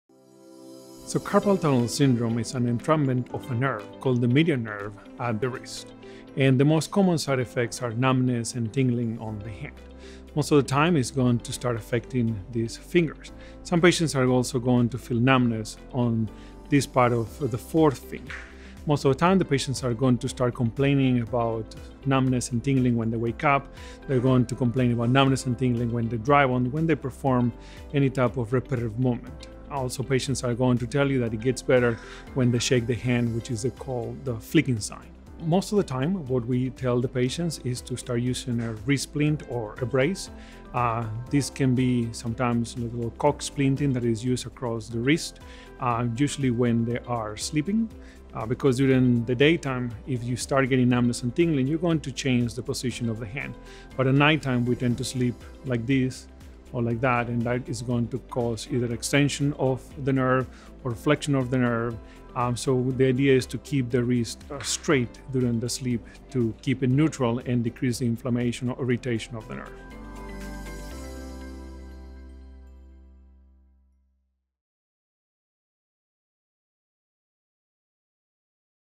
Medical Minute: Carpal Tunnel Syndrome
Dr. Gabriel Pantol, a board-certified neurologist at AdventHealth Tampa, explains Carpal Tunnel Syndrome, its symptoms, and what patients may be able to do to help reduce inflammation.